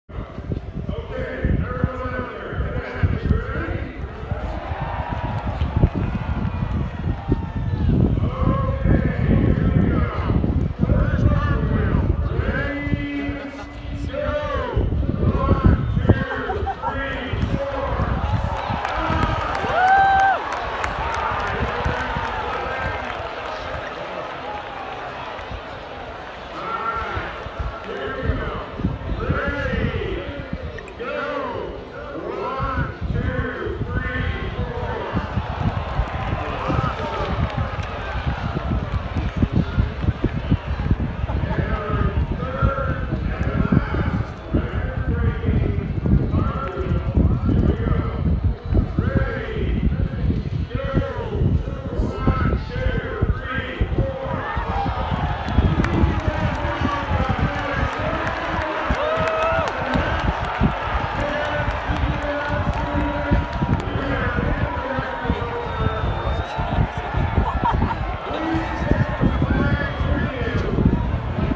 West Point Cartwheel
West Point Cadets attempt to set record for most simultaneous cartwheels!

Report is that they had 2300 people on the field today.  They counted everyone with turnstiles before entering the field.  Mostly West Point cadets, but also some faculty and families.